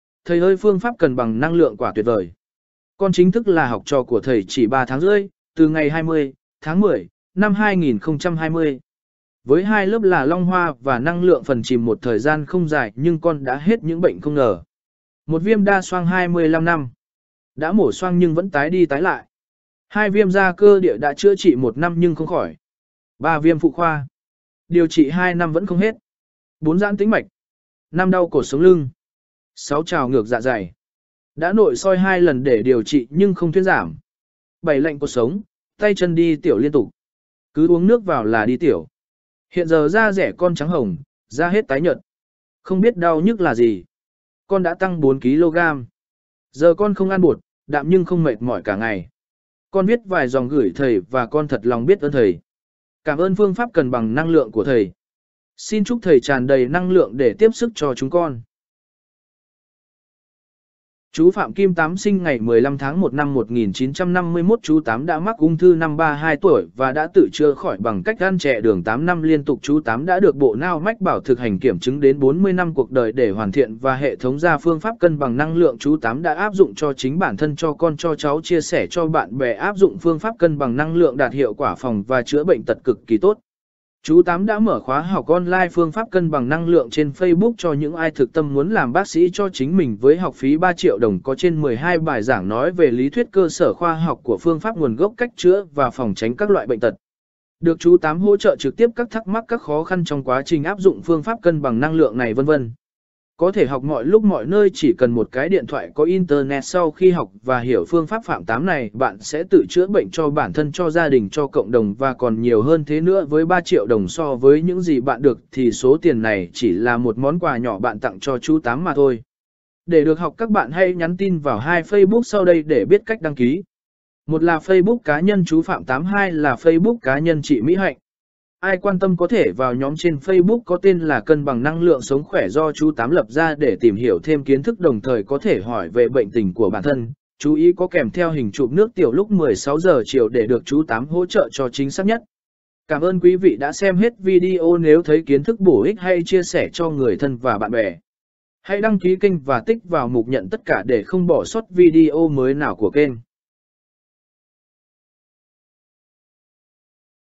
CHỮA KHỎI VIÊM ĐA XOANG,VIÊM DA CƠ ĐỊA VÀ NHIỀU BỆNH KHÁC CHỈ 3 THÁNG RƯỠI NHỜ PHƯƠNG PHÁP NÀY
CHỮA KHỎI 7 BỆNH CHỈ SAU 3 THÁNG RƯỠI KHI ÁP DỤNG PHƯƠNG PHÁP CÂN BẰNG NĂNG LƯỢNG PHẠM TÁM.
1.VIÊM ĐA XOANG 25 NĂM. ĐÃ MỔ XOANG NHƯNG VẪN TÁI ĐI TÁI LẠI
2.VIÊM DA CƠ ĐỊA.ĐÃ CHỮA TRỊ 1 NĂM NHƯNG KHÔNG KHỎI.
3.VIÊM PHỤ KHOA. ĐIỀU TRỊ 2 NĂM VẪN KHÔNG HẾT.
4.GIÃN TĨNH MẠCH.
5.ĐAU CỘT SỐNG LƯNG.
6.TRÀO NGƯỢC DẠ DÀY. ĐÃ NỘI SOI 2 LẦN ĐỂ ĐIỀU TRỊ NHƯNG KHÔNG THUYÊN GIẢM.
7.LẠNH CỘT SỐNG, TAY,CHÂN,ĐI TIỂU LIÊN TỤC. CỨ UỐNG NƯỚC VÀO LÀ ĐI TIỂU.